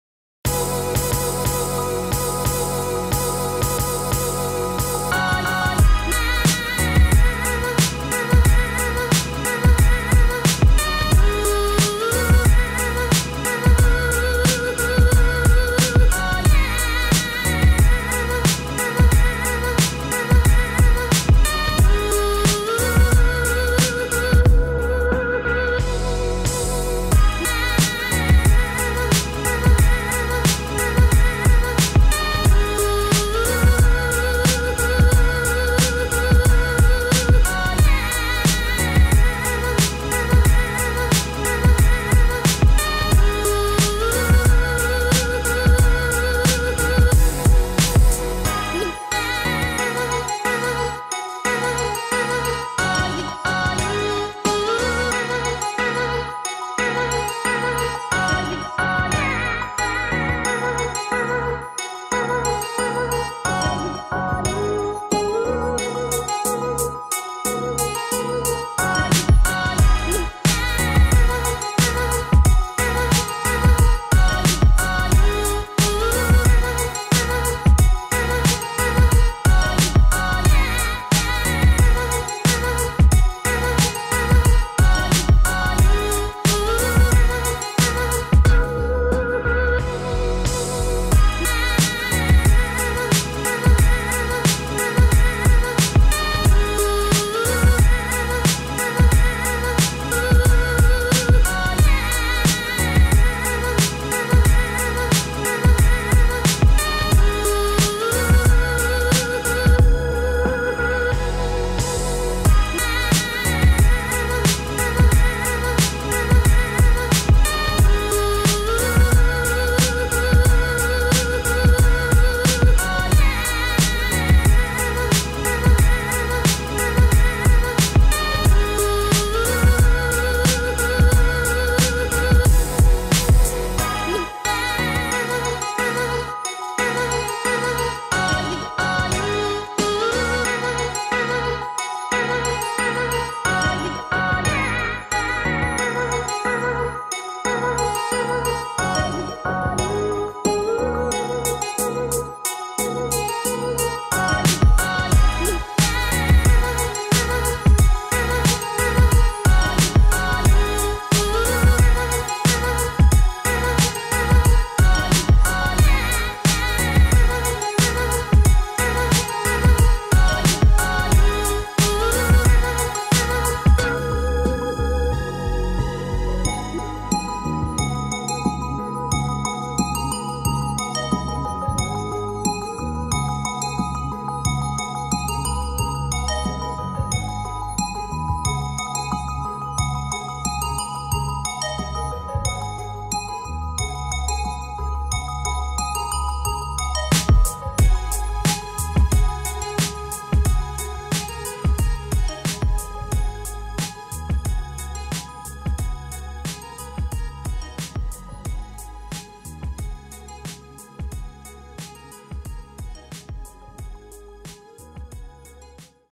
Sunday Freestyle | FPV Trick & Chill
Grab a cold drink and enjoy some beginner freestyle flying.